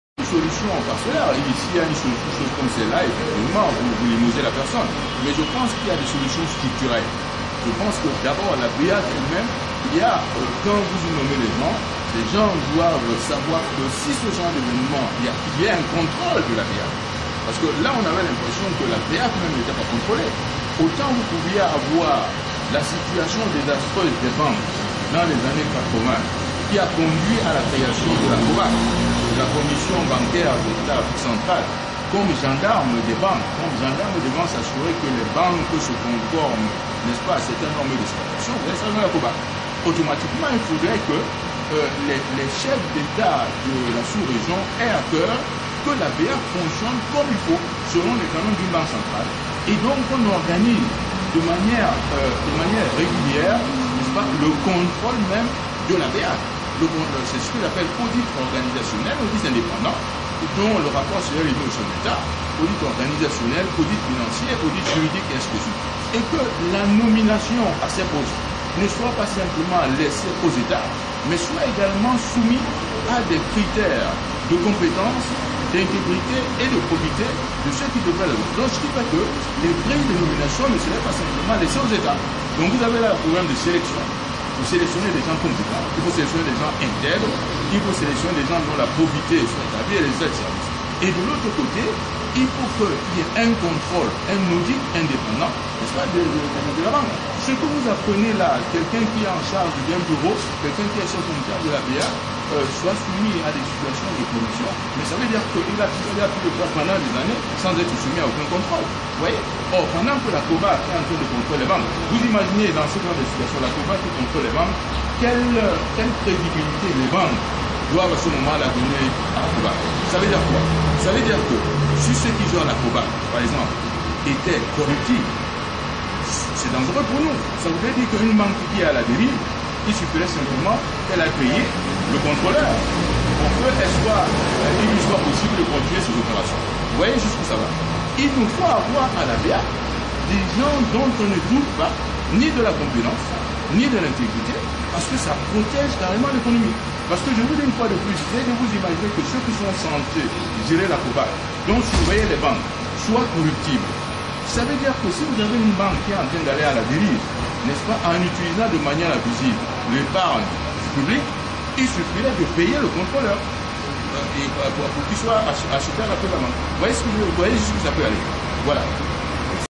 Foire Intersectorielle de Douala 2009
Interview de Christian Penda Ekoka au terme de l'Atélier N°3 (2)